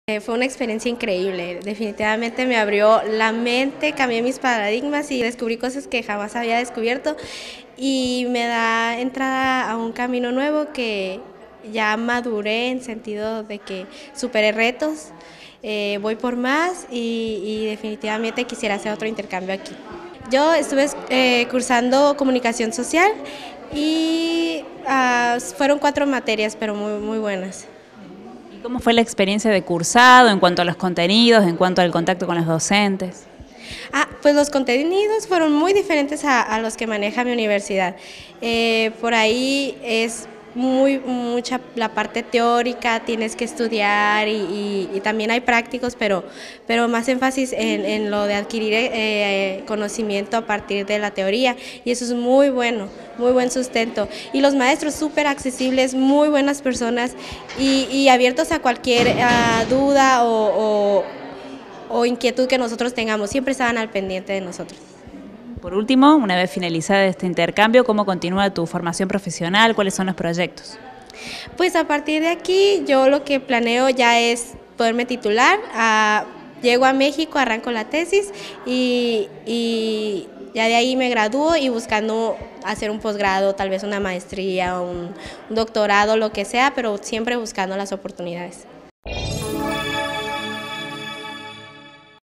UniRioTV - Despedida alumnos extranjeros - Guadalupe Lugo
GUADALUPE LUGO – ESTUDIANTE MÉXICO. PARTICIPARON DE PROGRAMAS DE MOVILIDAD ACADÉMICA.

Facebook.com/uniriotv
Twitter.com/uniriotv
Instagram.com/uniriotv
Web - uniriotv.unrc.edu.ar
o envianos un email a
UniRio TV
Universidad Nacional de Río Cuarto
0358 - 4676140